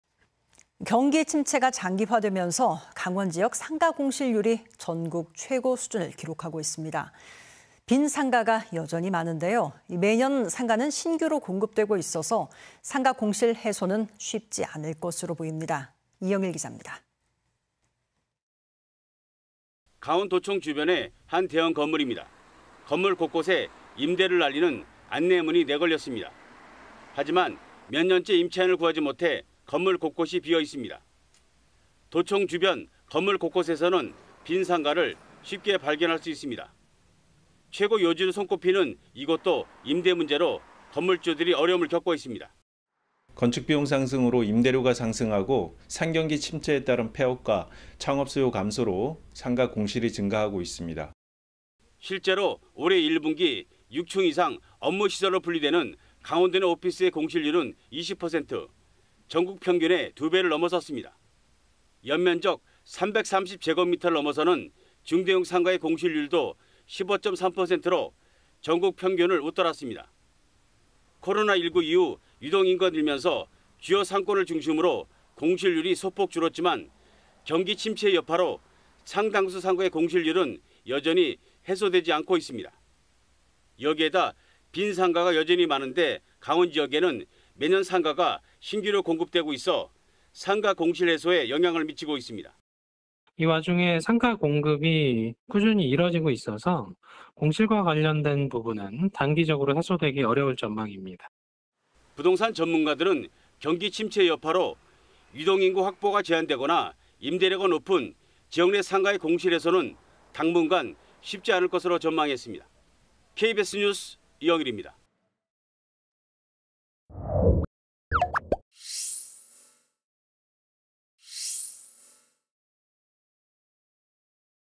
강원 상가 공실률 전국 최고…공실 해소 쉽지 않아 / KBS 2023.06.02.
[앵커]

 경기 침체가 장기화되면서 강원지역 상가 공실률이 전국 최고 수준을 기록하고 있습니다.

 빈 상가가 여전히 많은데, 매년 상가는 신규로 공급되고 있어 상가 공실 해소는  쉽지 않을 것으로 보입니다.

 이영일 기자의 보도입니다.

 [리포트]

 강원도청 주변의 한 대형 건물입니다.

 건물 곳곳에 임대를 알리는 안내문이 내걸렸습니다.

 하지만, 몇 년째 임차인을 구하지 못해 건물 곳곳이 비어 있습니다.

 도청 주변 건물 곳곳에서는 빈 상가를 쉽게 발견할 수 있습니다.

 최고 요지로 손꼽히는 이곳도 임대 문제로 건물주들이 어려움을 겪고 있습니다.

 [강문식/한국공인중개사협회 춘천시지회장 : "건축비용 상승으로 임대료가 상승하고, 상경기 침체에 따른 폐업과 창업 수요 감소로 상가 공실이 증가하고 있습니다."]

 실제로 올해 1분기 6층 이상 업무시설로 분류되는 강원도 내 오피스의 공실률은 20%.

 전국 평균의 2배를 넘어섰습니다.

 연면적 330제곱미터를 넘어서는 중대형 상가의 공실률도 15.3%로, 전국 평균을 웃돌았습니다.

 코로나19 이후 유동인구가 늘면서 주요 상권을 중심으로 공실률이 소폭 줄었지만, 경기 침체의 여파로  상당수 상가의 공실률은 여전히 해소되지 않고 있습니다.

 여기에다 빈 상가가 여전히 많은데, 강원지역에는 매년 상가가 신규로 공급되고 있어 상가 공실 해소에 영향을 미치고 있습니다.

 [윤지해/부동산114 리서치팀장 : "이 와중에 상가 공급이 꾸준히 이뤄지고 있어서 공실과 관련된 부분은 단기적으로 해소되기 어려울 전망입니다."]

 부동산 전문가들은 경기 침체의 여파로 유동인구 확보가 제한되거나, 임대료가 높은 지역 내 상가의 공실 해소는 당분간 쉽지 않을 것으로 전망했습니다.

 KBS 뉴스 이영일입니다.

 촬영기자:임강수